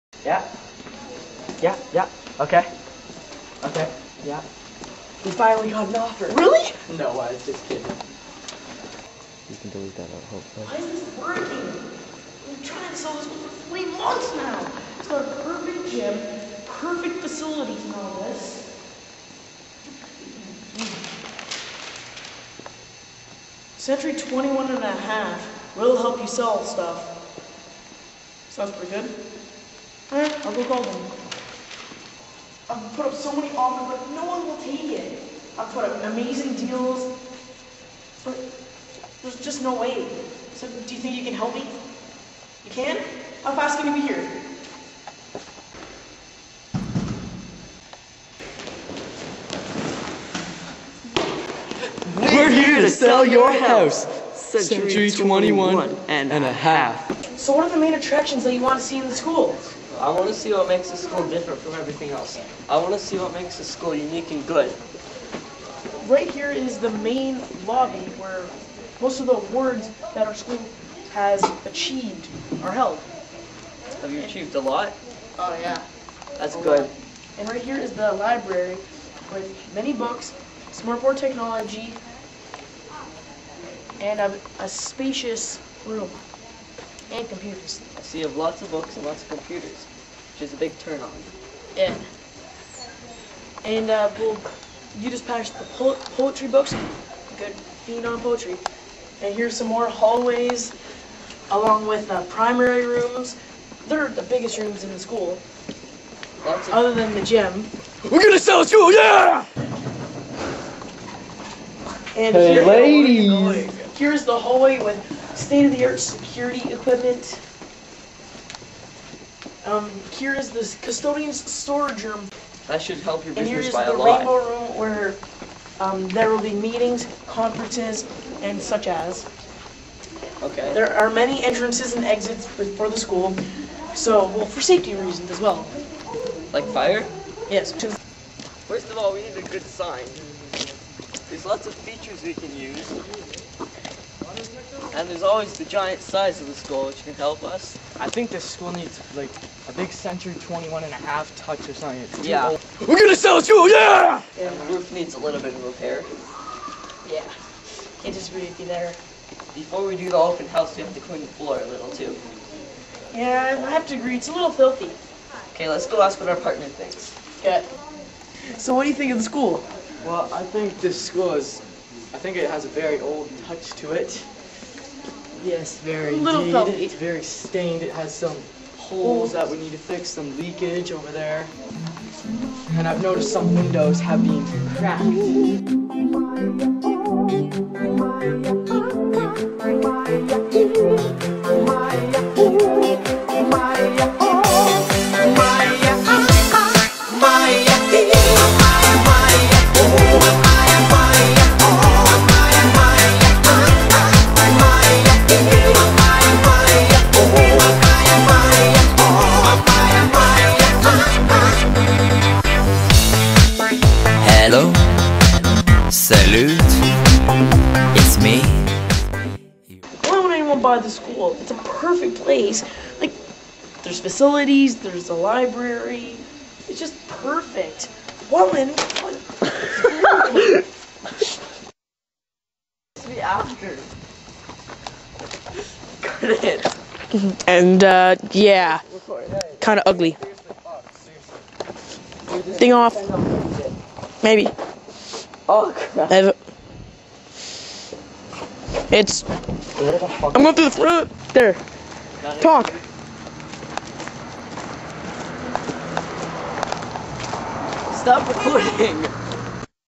nicholas wilson sell the school
project for selling nicholas wilson school(not real)